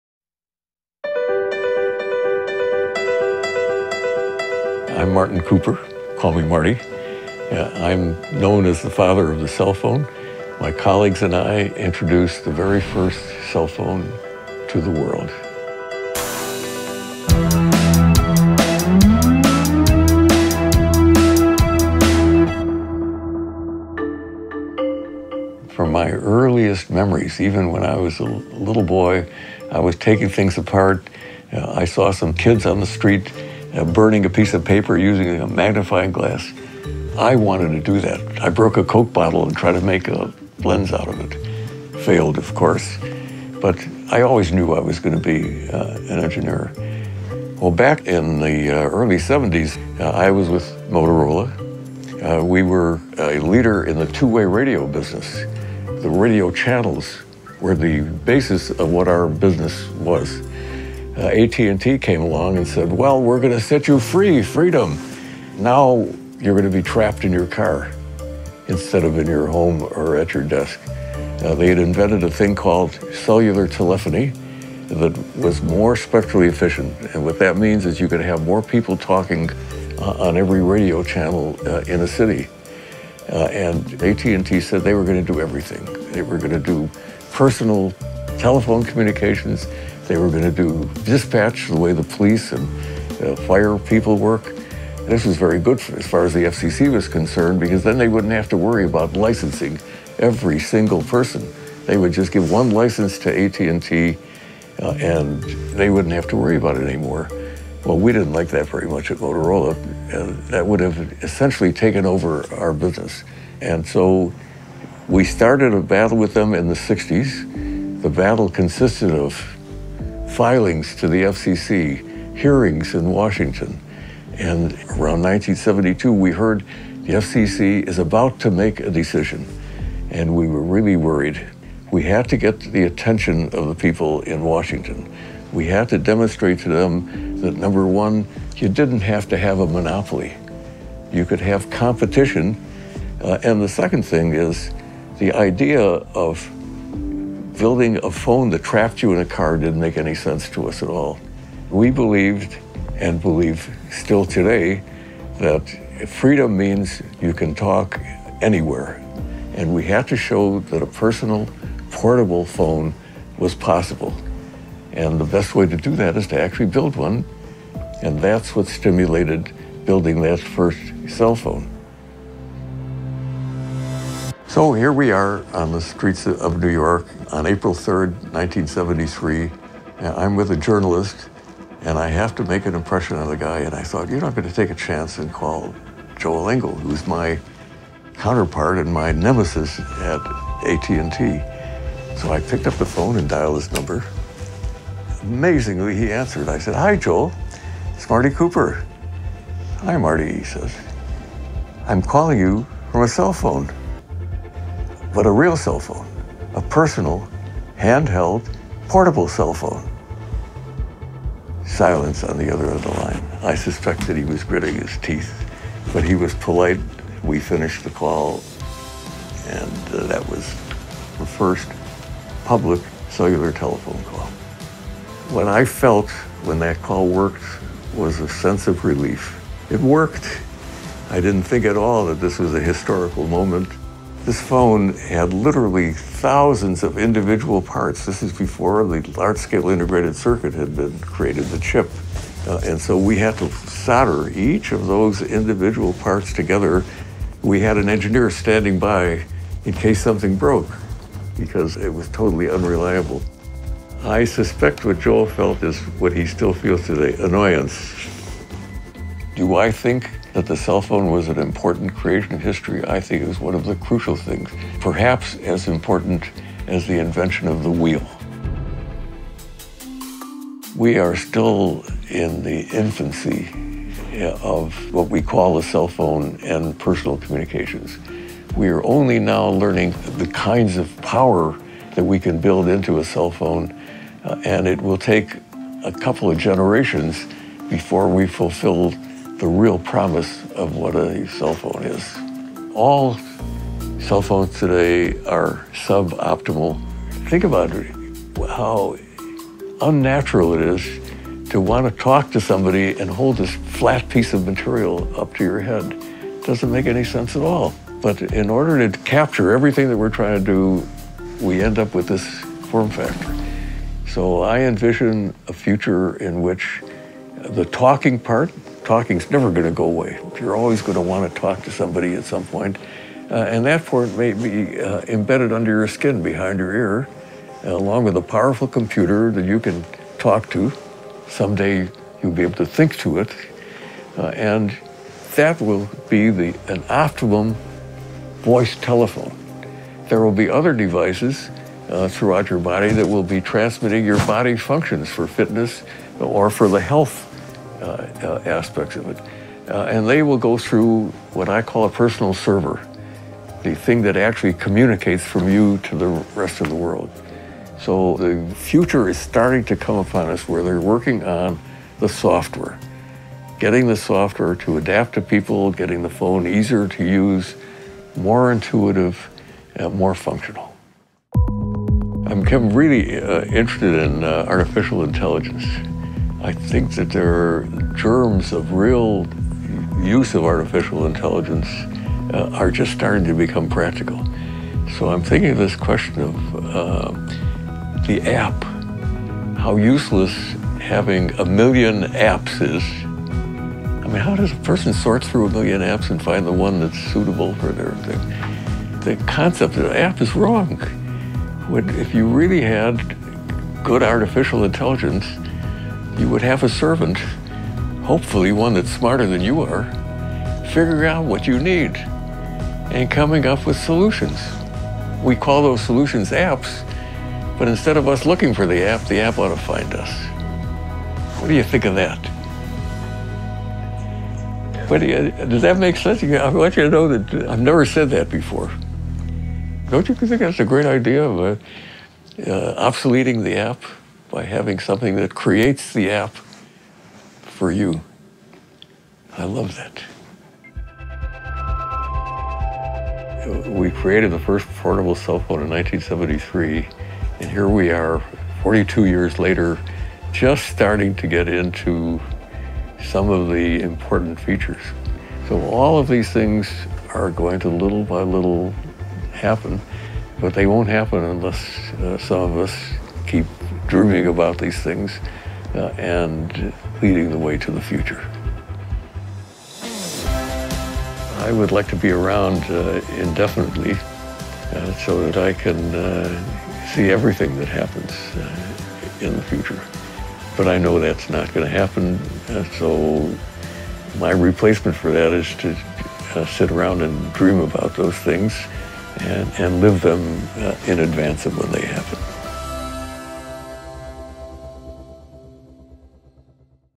携帯電話の父が語る本当の自由
1960年代から始まったAT&amp;Tとモトローラの携帯電話開発抗争。1973年にその軍配はモトローラに上がった。あれから42年、自動車以来の大発明として携帯電話は常に進化している。しかし86歳になった彼は言う。まだまだ携帯電話は幼児期にあると…。世界初の携帯電話を開発したマーティン・クーパーがその歴史を振り返ると共に、その未来をも語る。「君だけのアプリを開発して、アプリを時代遅れにしないとね」

原題：Meet Marty Cooper, the Forefather of Your Cell Phone